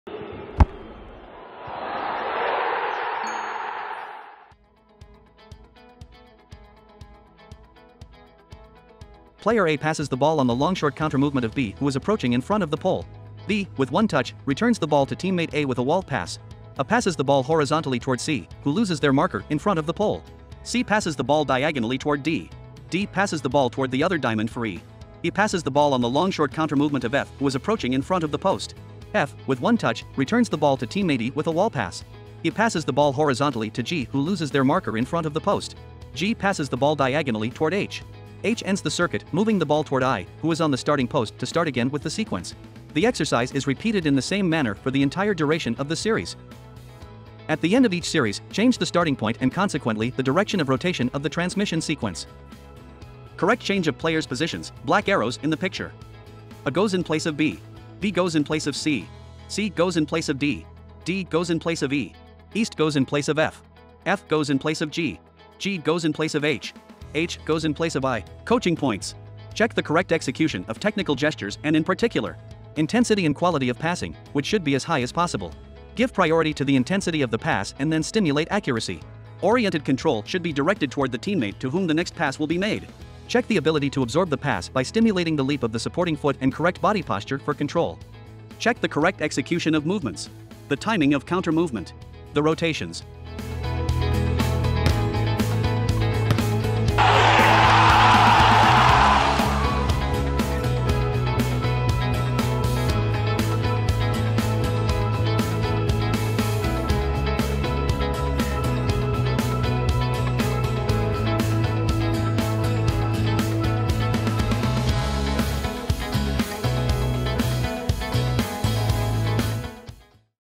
Soccer drill: Combination technique circuit - Barcelona FC
⚽ Combination technique circuit - Barcelona FC

🎯 Goal: passing

ℹ Coaching points:
✔ Check the correct execution of technical gestures and in particular:
- intensity and quality of passing, which should be as high as possible
- give priority to the intensity of the pass and then stimulate accuracy
- oriented control should be directed toward the teammate to whom the next pass will be made
- check the ability to absorb the pass by stimulating the leap of the supporting foot and correct body posture for control
✔ Check the correct execution of movements:
- the timing of countermovement
- the rotations